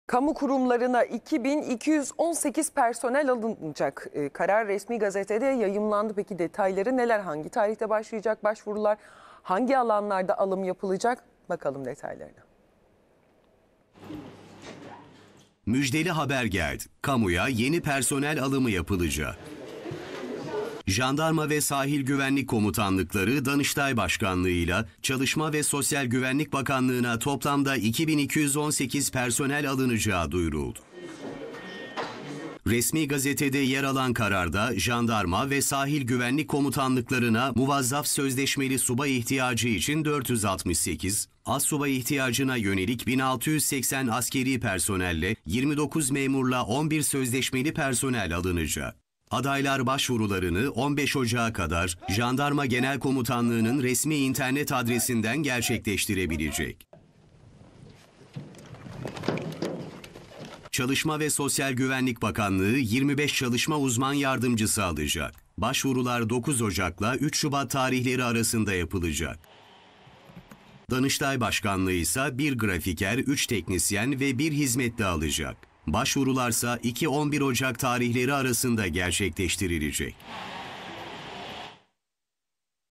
Kamu kurumlarına personel alımı! Başvurular ne zaman ve şartlar neler? | A Haber
Jandarma ve Sahil Güvenlik Komutanlıkları ile Çalışma ve Sosyal Güvenlik Bakanlığı ve Danıştay Başkanlığı personel alımı yapılacağını duyurdu.
Resmi Gazete'de yayımlanan ilanlara göre, Jandarma ve Sahil Güvenlik Akademisi Başkanlığında eğitim-öğretim görecek, Jandarma Genel Komutanlığının muvazzaf/sözleşmeli (erkek/kadın) subay ve Sahil Güvenlik Komutanlığının sözleşmeli (erkek/kadın) subay ihtiyacına yönelik 468 askeri personel alınacak.

Jandarma Genel Komutanlığının muvazzaf/sözleşmeli (erkek/kadın) astsubay ve Sahil Güvenlik Komutanlığının sözleşmeli (erkek/kadın) astsubay ihtiyacına yönelik de 1680 askeri personel alımı yapılacak.

Başvuru şartları ve aranan niteliklere ilişkin bilgiler " adresindeki başvuru kılavuzunda yer alacak.